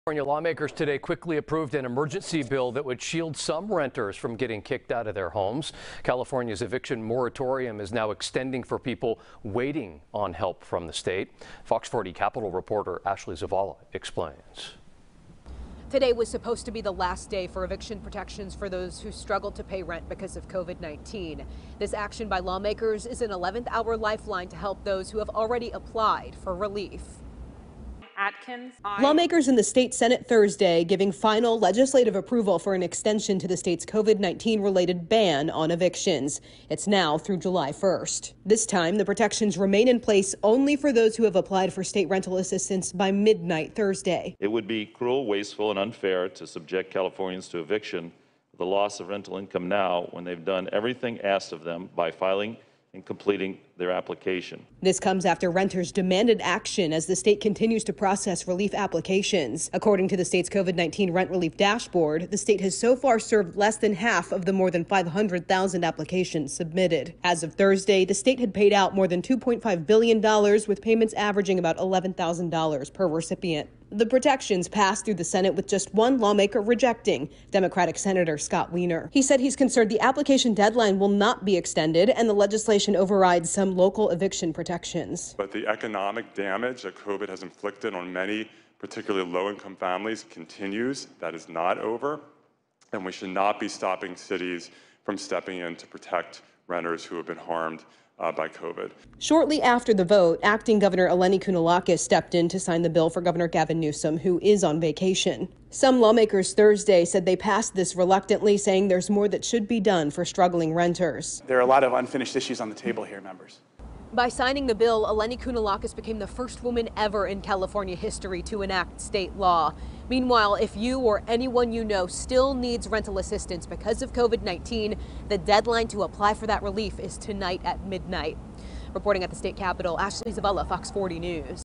New California law extends eviction ban for some renters
Ashley Zavala reports